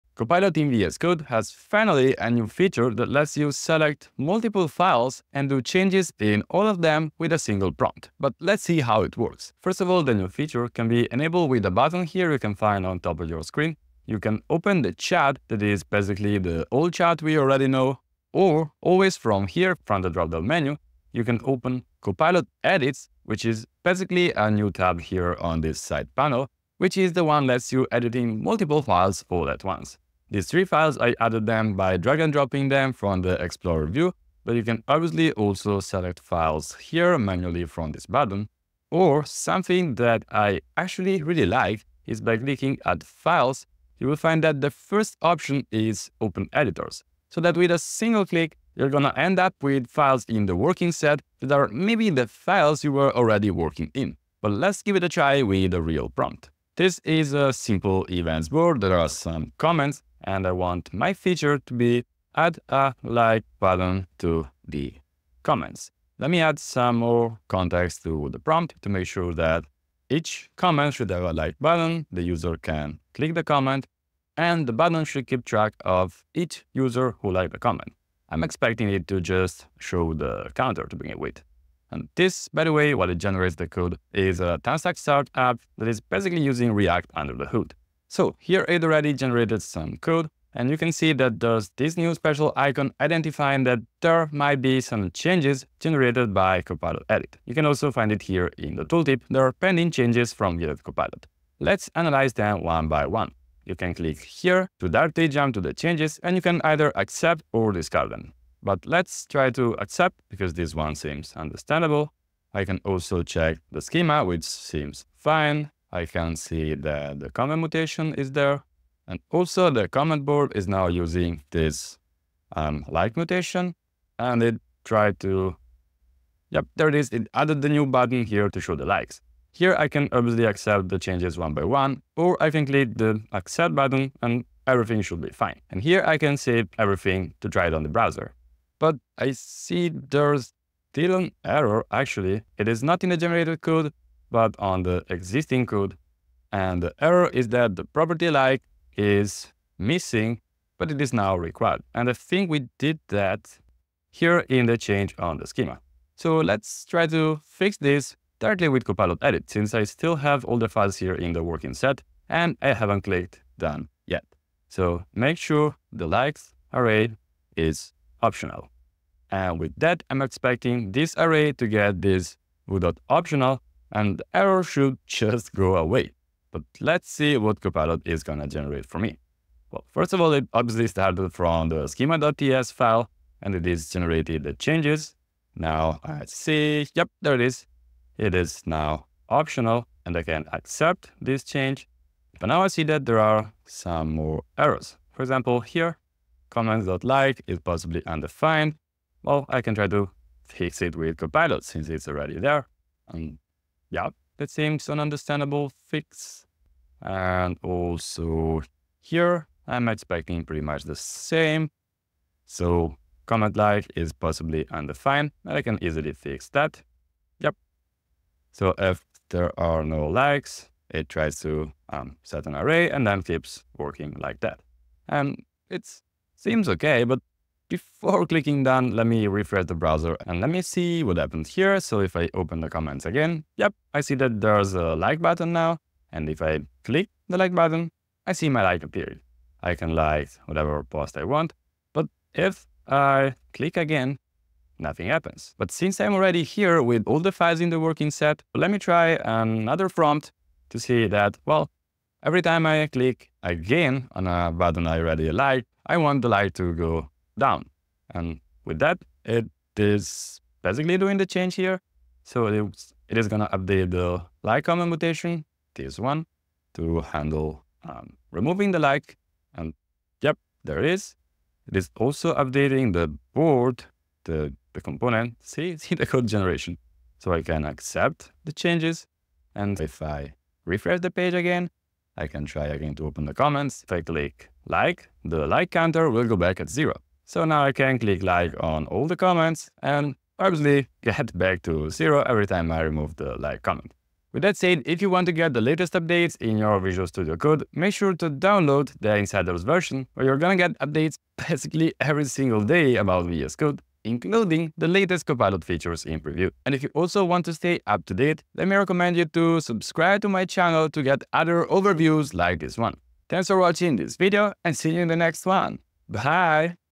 Copilot Editing Multiple Files in Visual Studio Code
Generating changes in multiple files with a single prompt is now supported with GitHub Copilot in Visual Studio Code!

And who am I to not give it a try and share a demo of it?